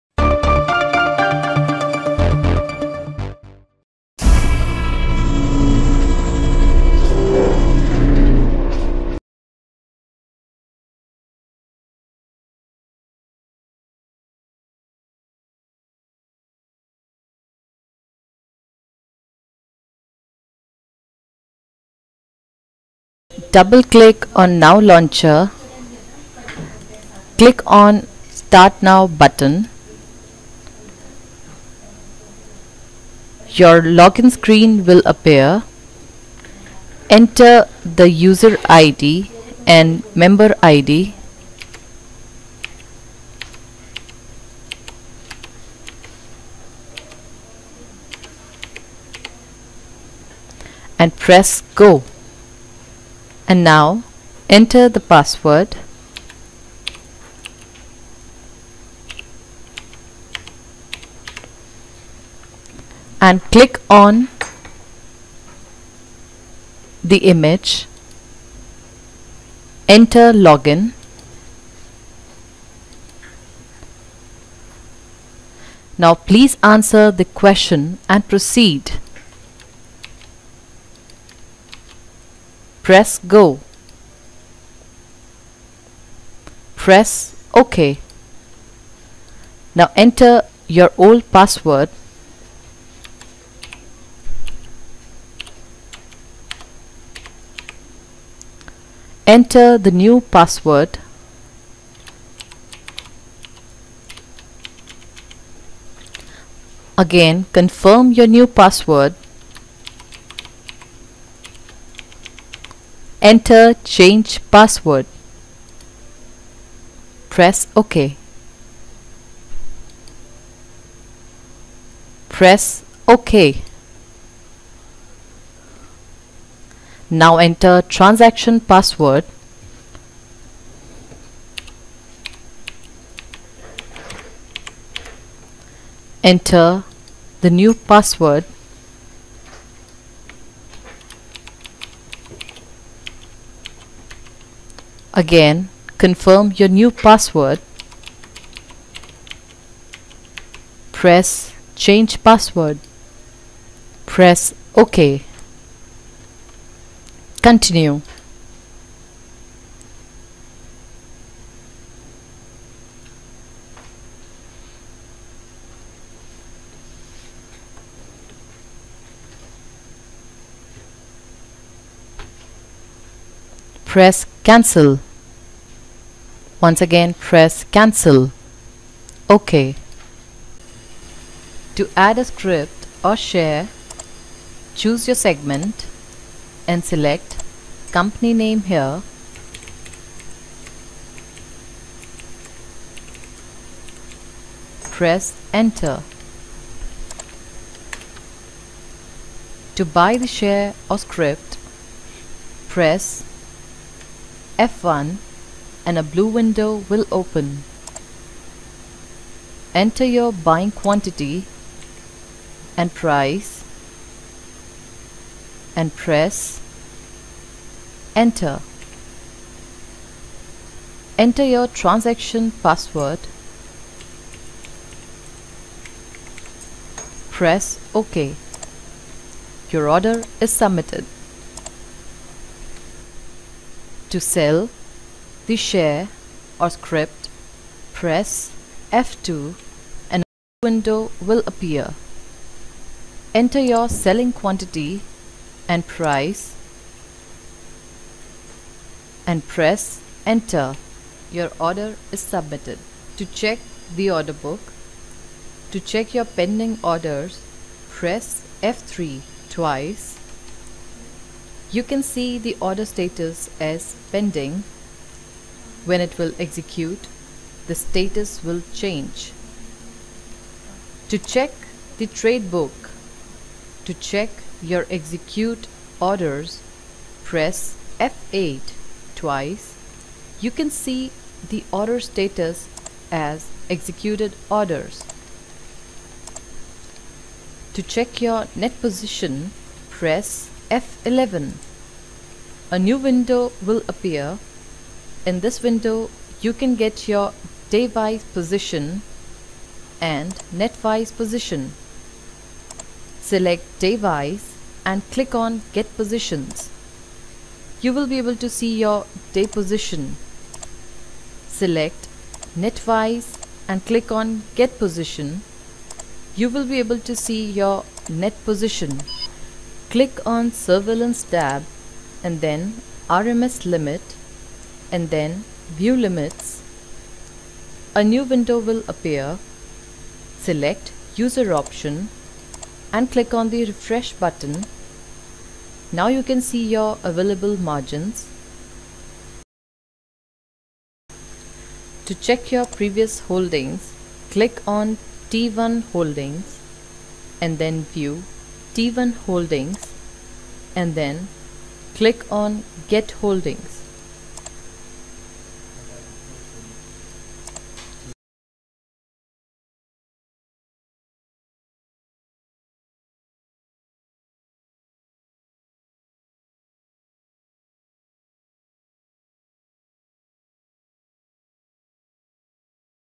How to Operate & Use NSE NOW Stock Trading Software
Learn How to Operate and Use NOW Trading Software in easy steps uploaded